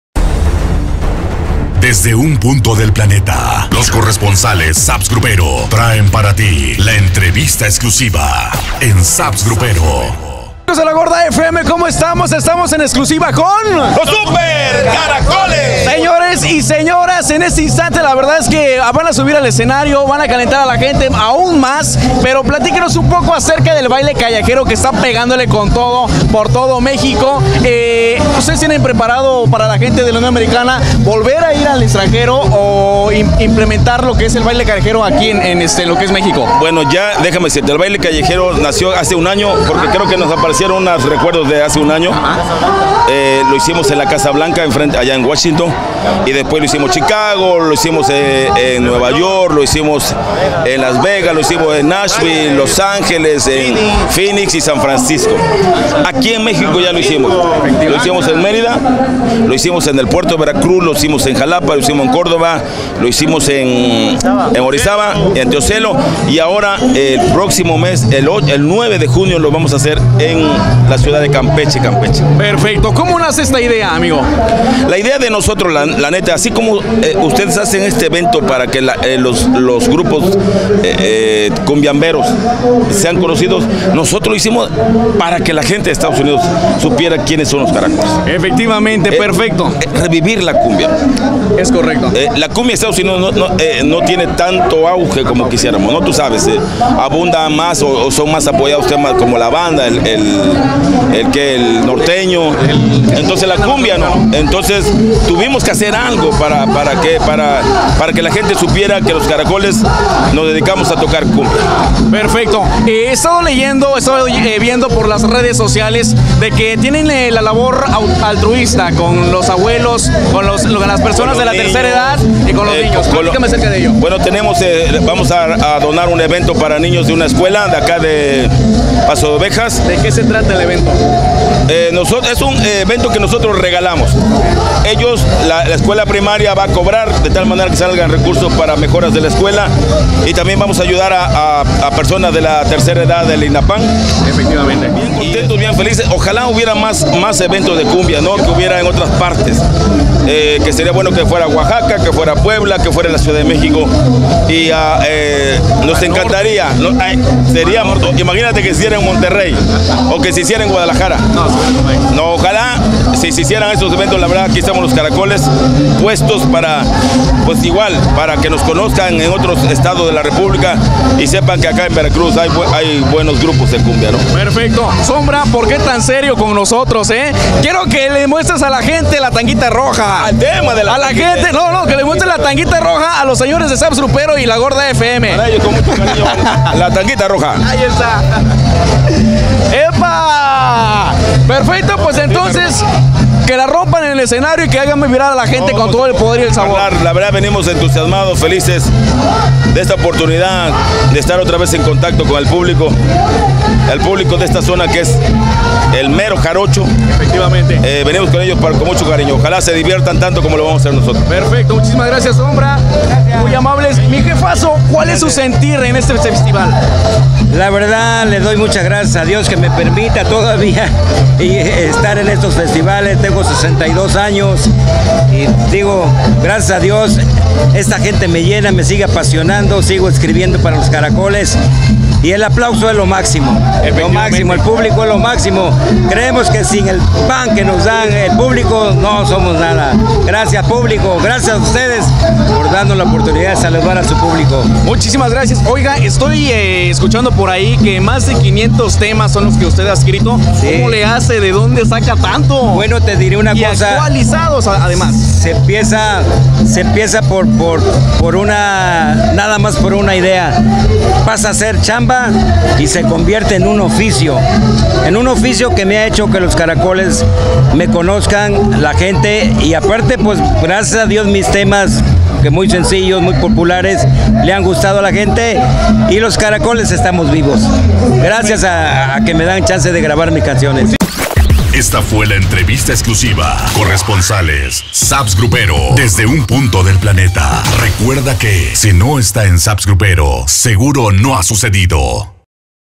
Los Súper Caracoles en Unidos Por La Cumbia Boca Del Río 2019
Si hay un estado que vibra, siente y hace suyas las notas musicales de la alegría, esa entidad sin duda es Veracruz, misma que recibió a brazos abiertos la segunda fecha del Festival Unidos Por La Cumbia 2019.